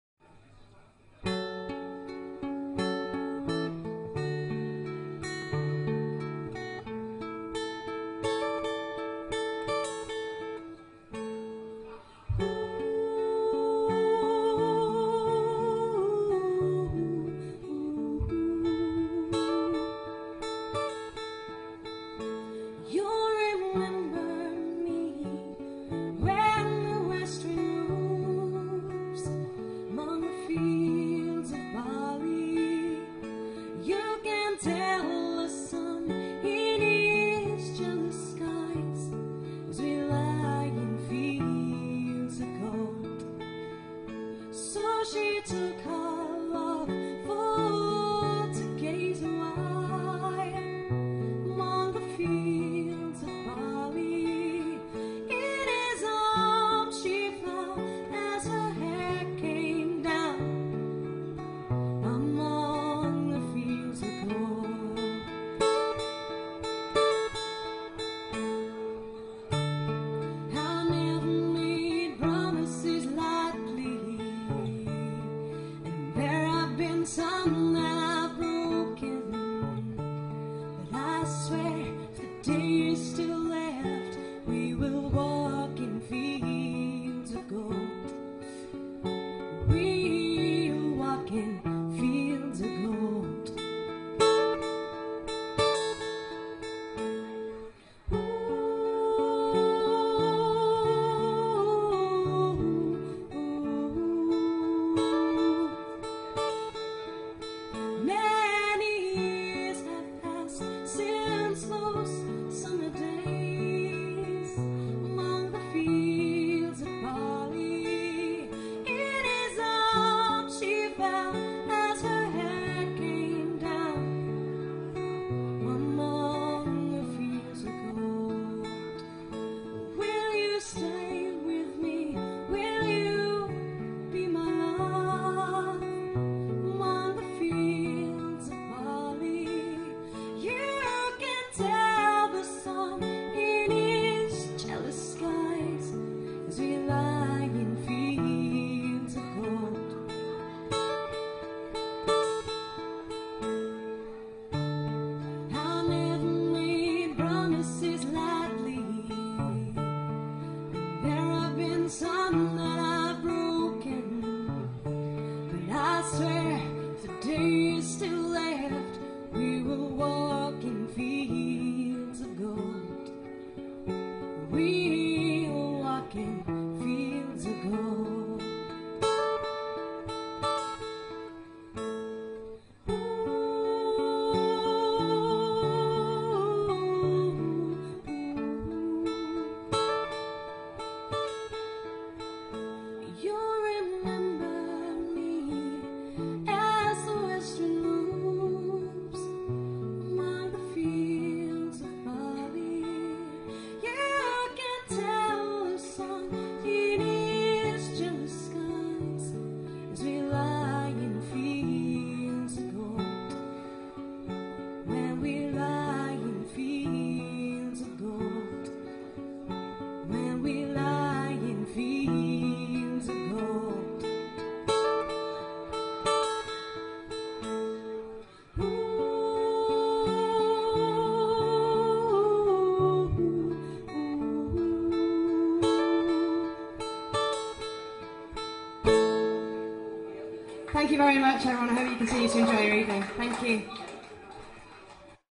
Female Acoustic Guitar Wedding Singer Manchester - Kathleen Amanda
A versatile performer suitable to provide high standard entertainment at any venue!
Experience of performing as a background instrumentalist in restaurants and hotel as well as performing lively and vibrant performances as a main feature entertainer in various bars, clubs, and private parties such as birthdays and weddings.